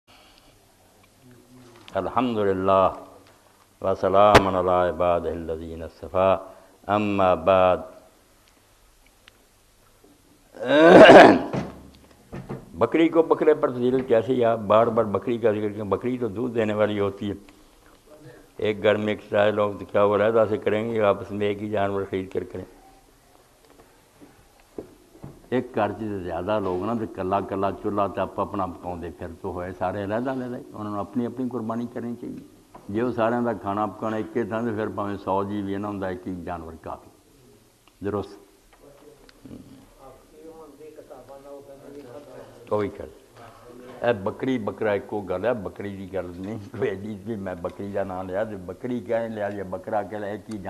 2008 11 28 Agar ghar walay alag alag khana pakana Karen to phir sab apni alag alag qurbani Karen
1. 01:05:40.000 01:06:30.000
 Agar ghar walay alag alag khana pakana Karen to phir sab apni alag alag qurbani Karen gay.
 Bakra ya bakri ki qurbani.